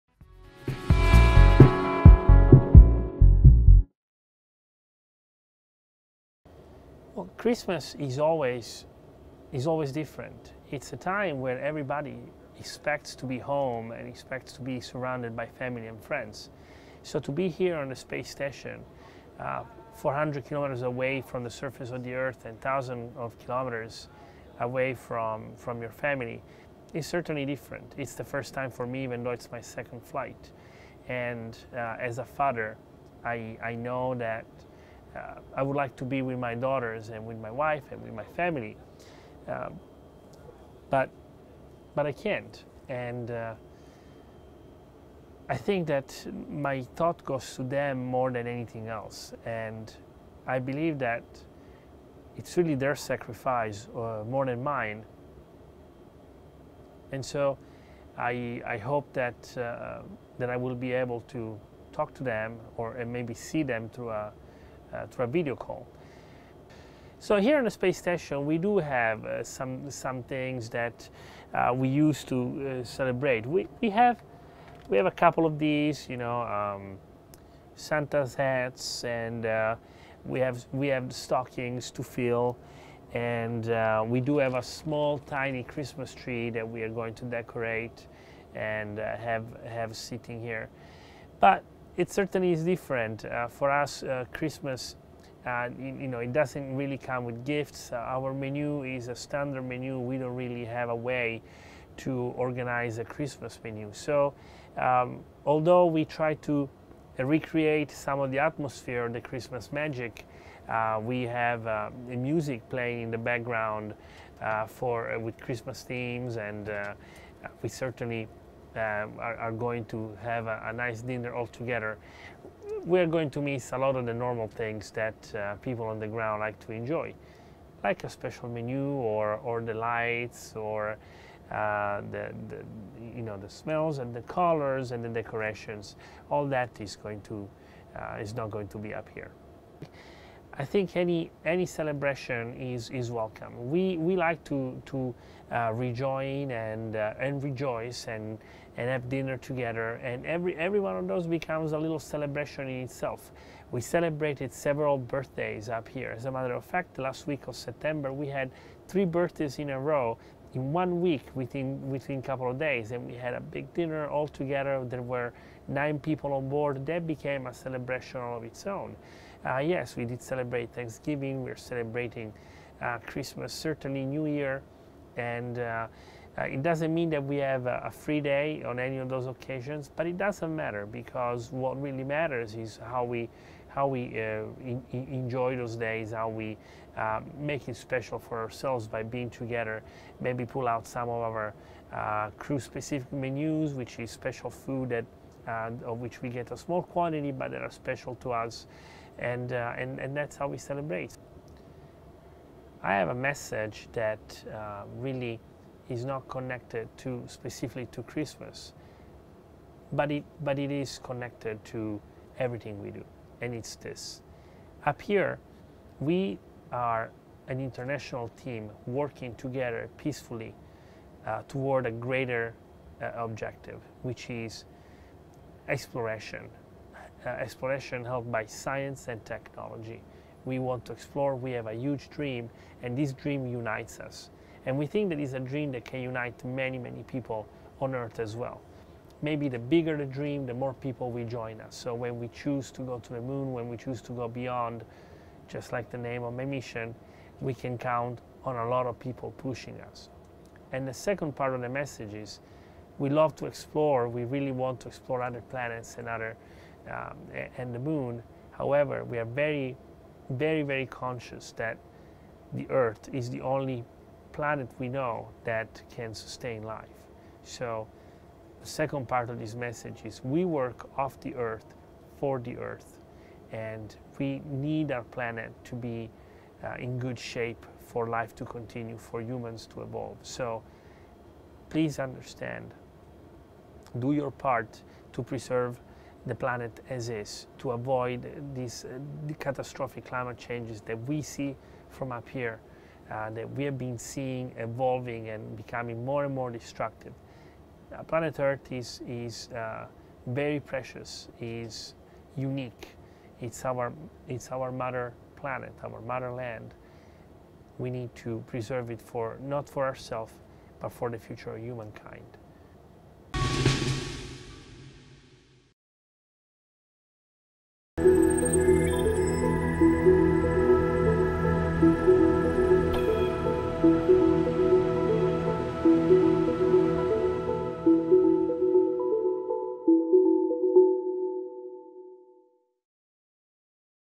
Mission Beyond: Luca’s Christmas message
ESA astronaut Luca Parmitano shares a message with everyone back on Earth as he and his crew prepare to celebrate Christmas on board the International Space Station.

Though Beyond is Luca’s second mission to the Space Station, 2019 will be the first time he spends Christmas in orbit. In this message Luca reflects on the things he will miss, including his family and all the lights, colours, food and smells that go along with the holiday season.

As Luca reflects on Christmas on Earth, he also reflects on the next step in space exploration and the importance of preserving our home planet for future generations.

Luca is the third European and first Italian commander of the International Space Station. He was launched for his second mission from the Baikonur Cosmodrome in Kazakhstan on 20 July 2019 and will return to Earth in February 2020.

While in orbit, Luca is supporting more than 50 European experiments across a broad range of disciplines. This vital research in microgravity helps us progress medicine and technology on Earth while preparing us to explore even farther in space.